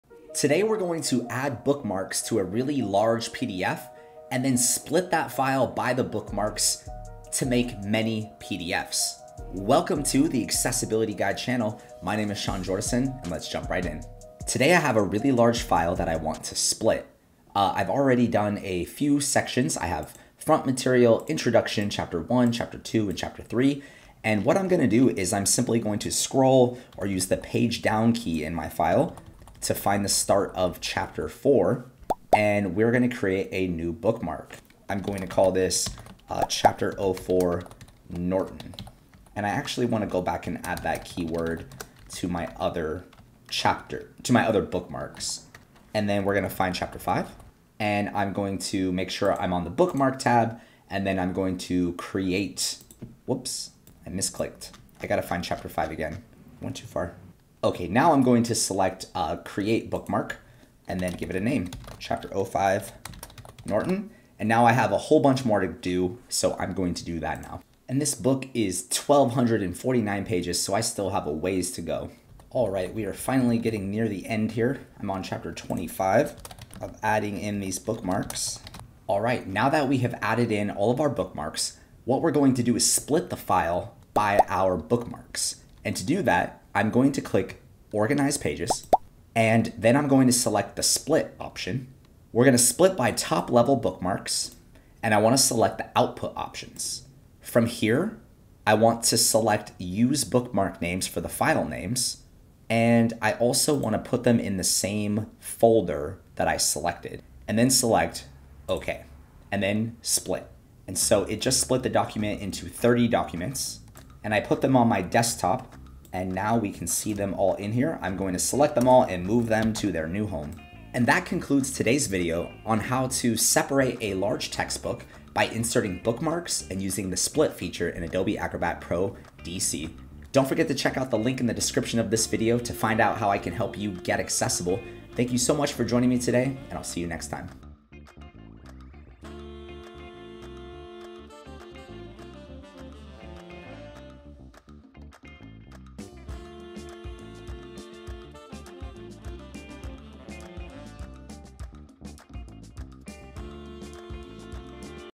How to add bookmarks and split a PDF using adobe acrobat pro
This video will walk users through how to split a PDF using bookmarks in Adobe Acrobat Pro DC